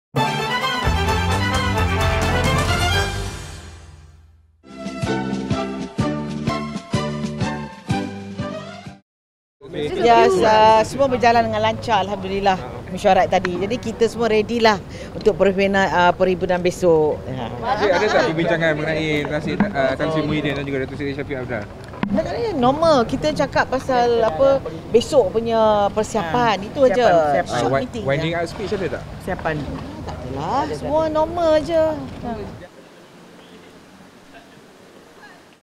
Shahrizat: It was just a normal meeting
Wanita Umno chief Datuk Seri Shahrizat Abdul Jalil explains about the atmospheres and the nature of the meeting of the party's supreme council members.